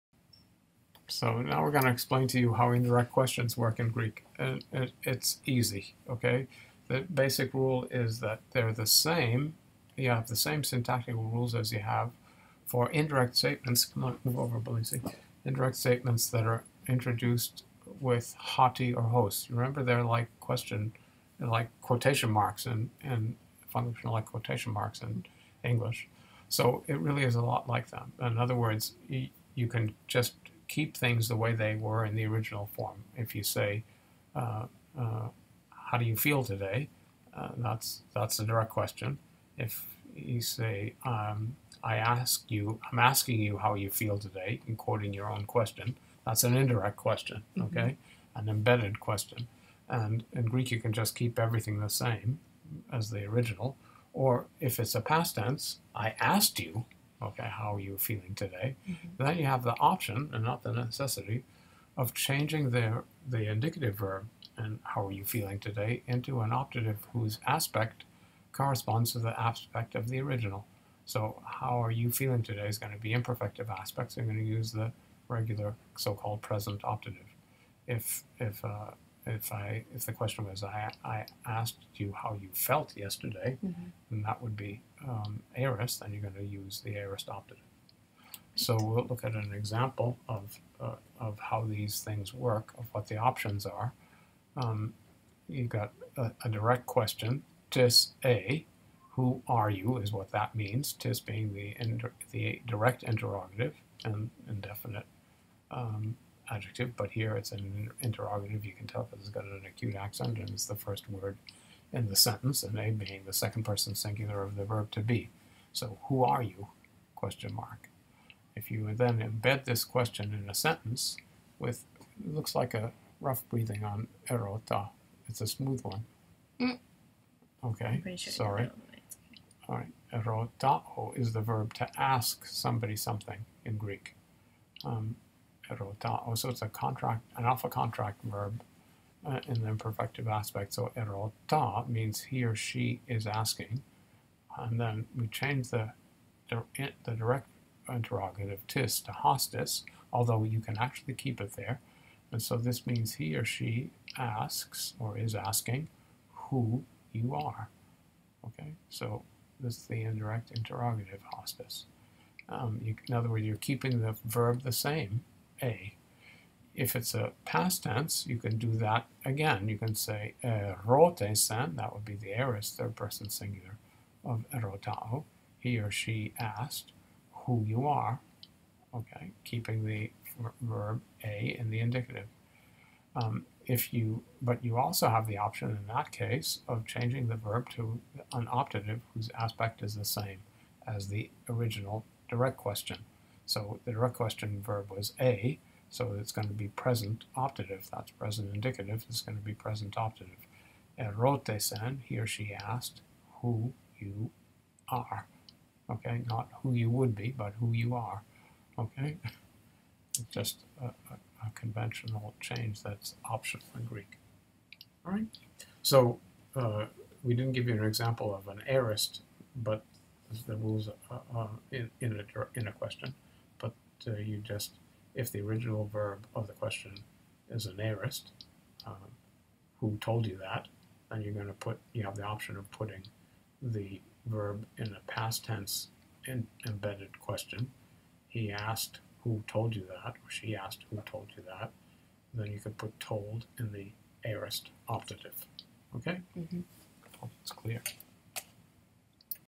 Learn Ancient Greek: 104_Unit 18 Indirect questions
You might never have considered what an 'indirect question'is, but it's a pretty frequent phenomenon in Ancient Greek! In fact, it works very similarly to the indirect statement, or embedded speech, that you learned in Unit 16. Professor Muellner explains what indirect questions are, when you might find them, and how to construct them in Ancient Greek. Pages 526-528 in the Hansen & Quinn textbook provide helpful examples and paradigms.